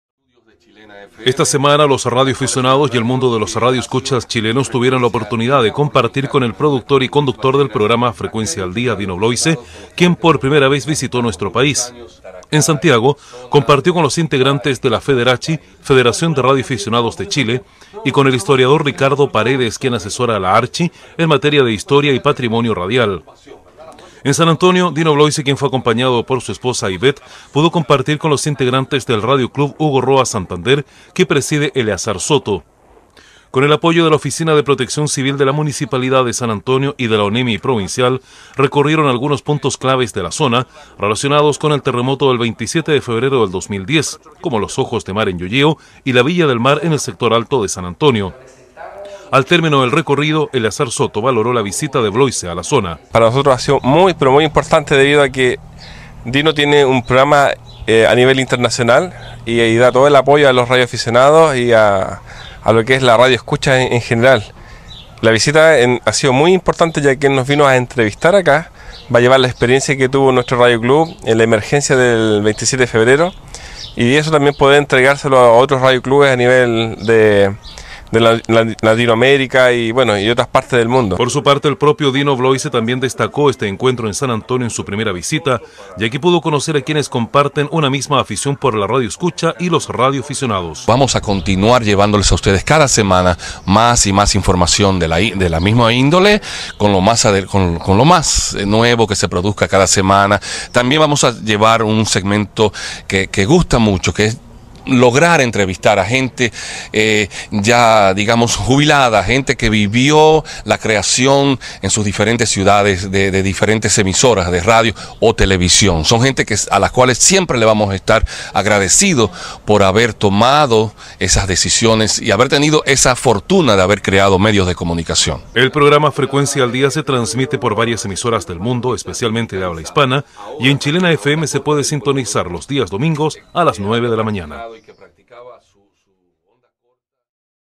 El radioaficionado Dino Bloise (Frecuencia al día) visita Chile con la ONEMI
El conductor del programa radiofónico Frecuencia al día, que se emite en diferentes emisoras de todos los continentes visita la emisora de radio Chilena FM.

Durante la estancia en el país fue acompañado por miembros de la Federación de radioaficionados de Chile (FEDERACHI), Radio Club Hugo Roa Santander y de ONEMI, así como algunos periodistas.